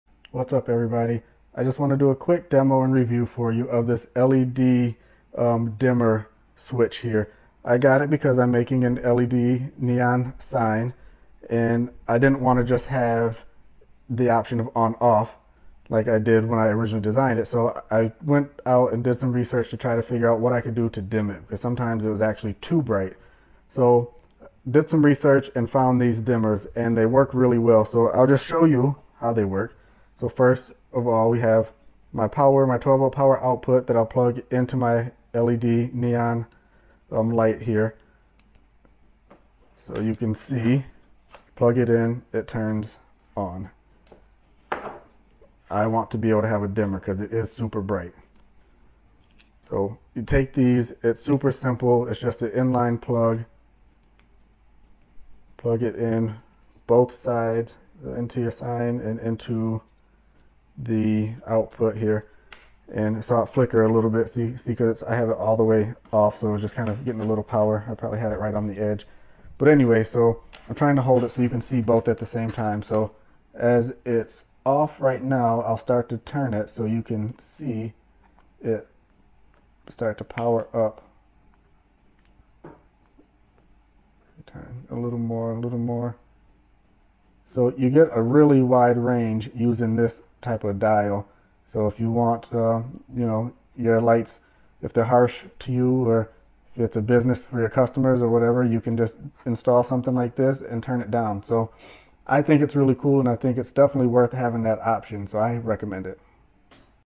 LED Dimmer Switch for Neon Signs: Full Review & Demo
In this video, I review a simple yet effective LED dimmer switch that I use for my DIY neon sign projects. This dimmer allows you to adjust the brightness of your LED lights with a wide range, making it perfect for various settings, whether at home or in a business. It’s easy to install and works well to control harsh light. Watch the demo to see how this dimmer switch performs with LED neon signs!

Love my content? Help me create more by buying me a coffee!